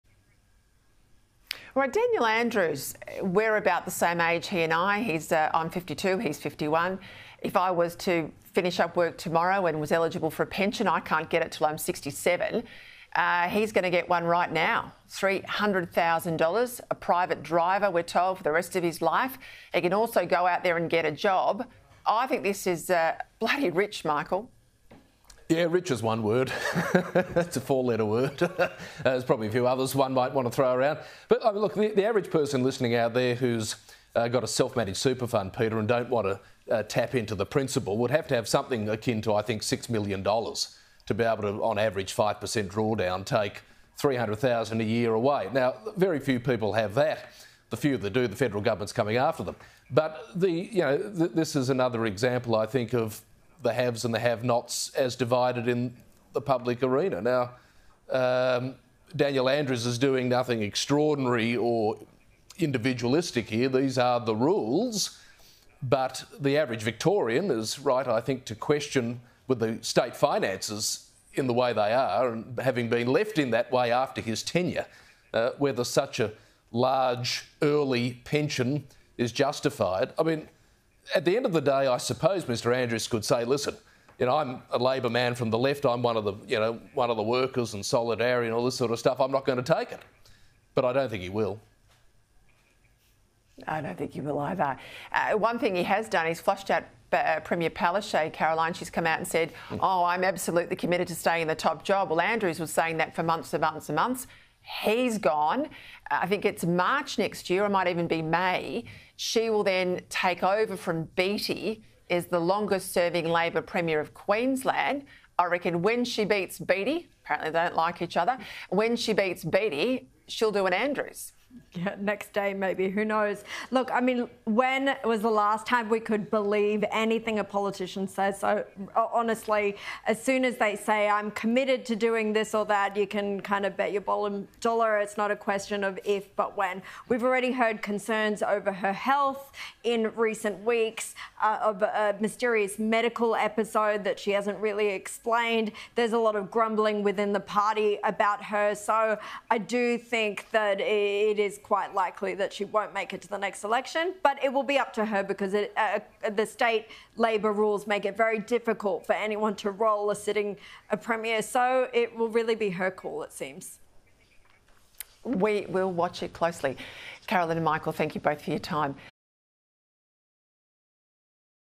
‘Grumbling’ within Annastacia Palaszczuk’s party
Sky News Senior Reporter Caroline Marcus says there is “grumbling” within Queensland Premier Annastacia Palaszczuk’s Labor party.

The Queensland premier recently said she would continue in her position despite former Victorian premier Daniel Andrews stepping down on Wednesday.

“When was the last time we could believe anything a politician says?” Ms Marcus told Sky News host Peta Credlin.

“So, honestly, as soon as they say, ‘I’m committed to doing this or that,’ you can kind of bet your dollar it's not a question of if but when.

“We’ve already heard concerns over her health in recent weeks due to a mysterious medical episode that she hasn’t really explained.”